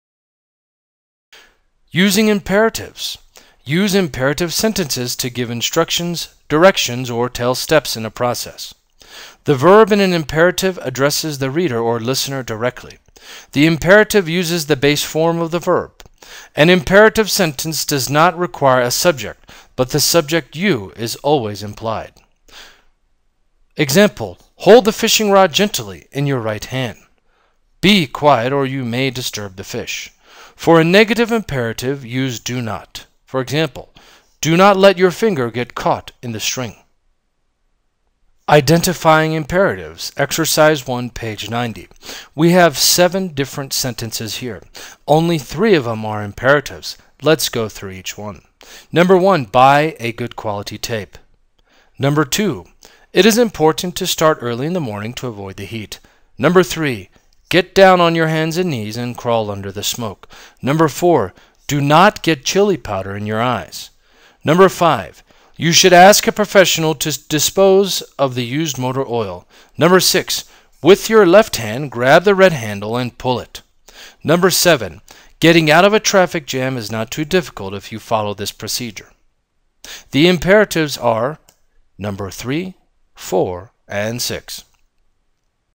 Scientific English | C4-L5 | Imperatives in a process paragraph
المقررات المفتوحة - Scientific English ENG 110
Unit 4 - Lecture 5 : Imperatives in a process paragraph
Using Imperative
Identifying Imperatives